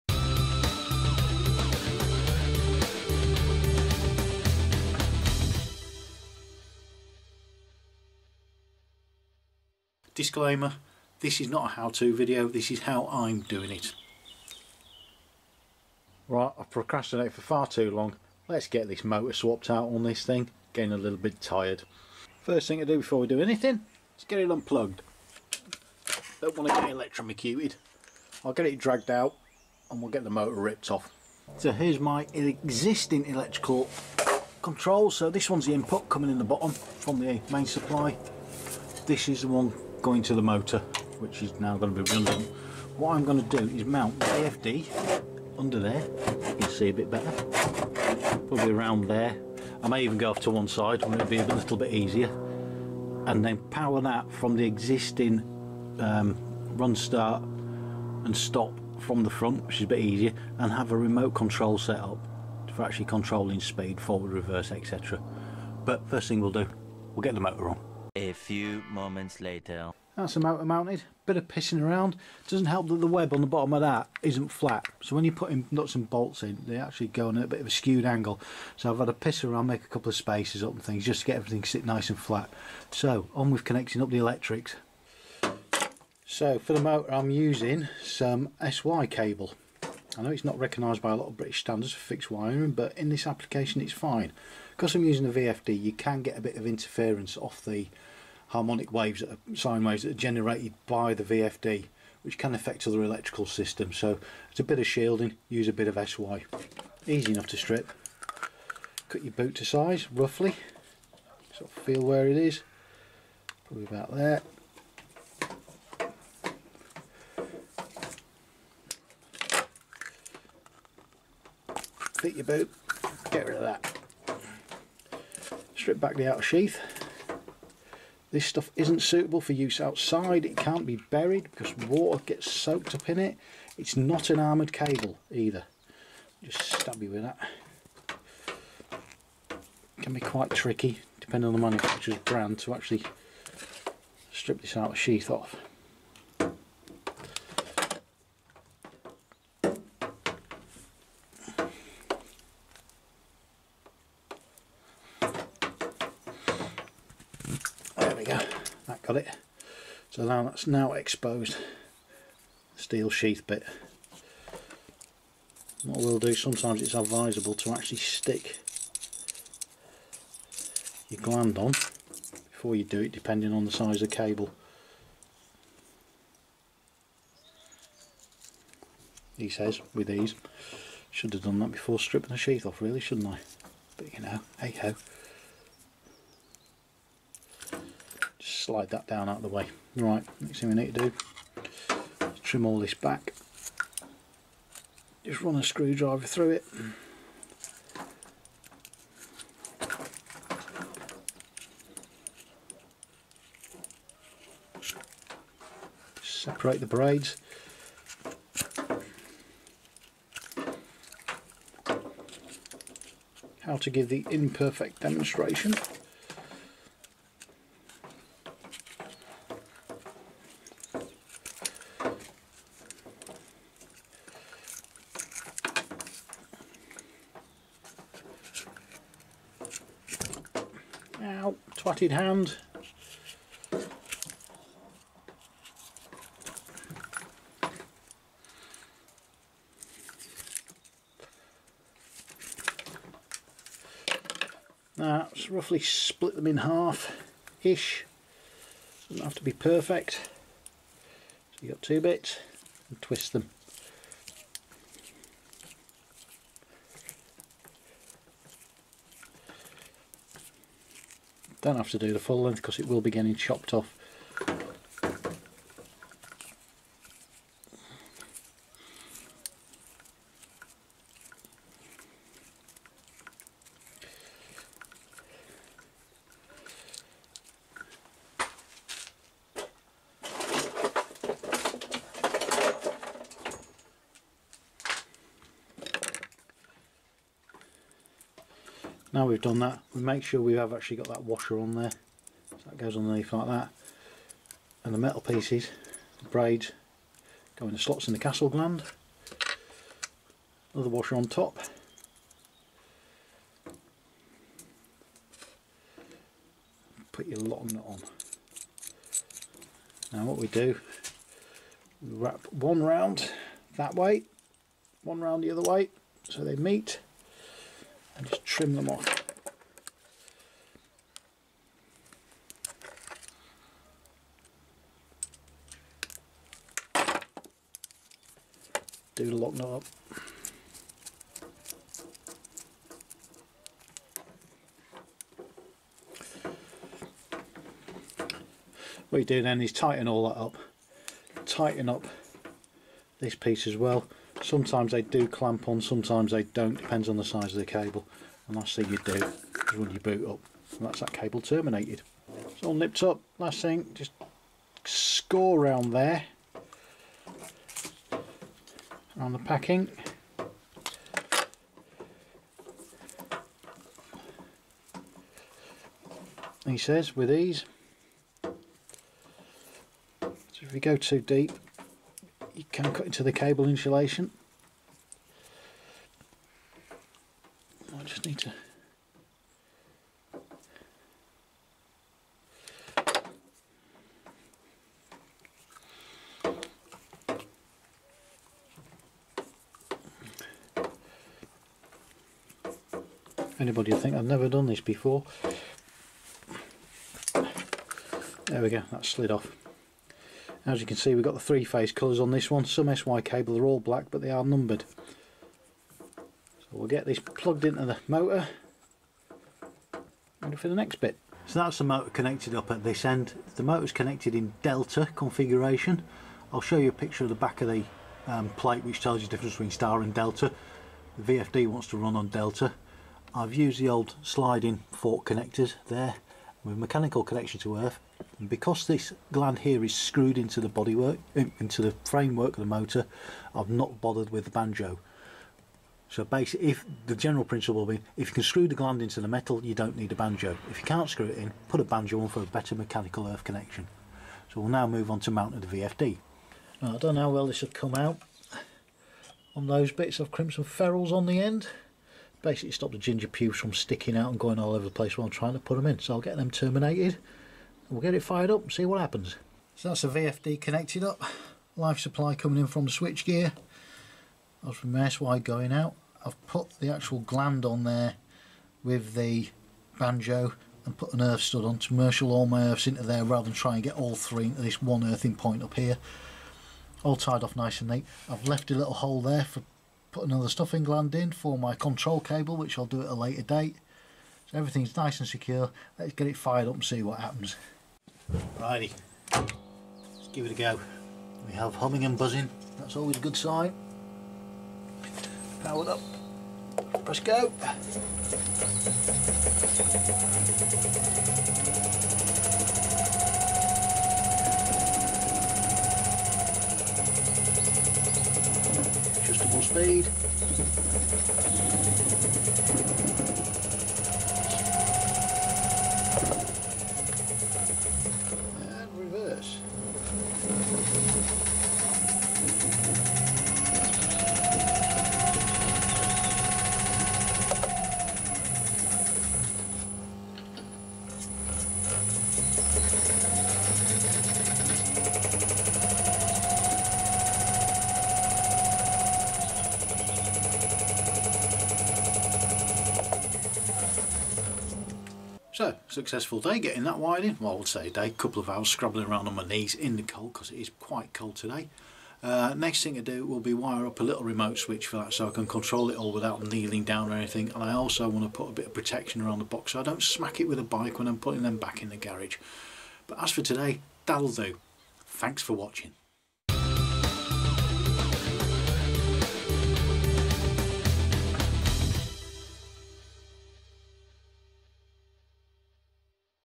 Myford ML7 Three Phase Motor and VFD Installation
The motor on my Myford ML7 is a little tired and keeps stalling. It's not the original motor for the lathe and the old VIR insulation is crumbling off and the windings are pretty much shot.

I bought a VFD and three phase motor a couple of years ago and have finally got round to fitting it.

This is not a how to, just how I did it if you need any ideas.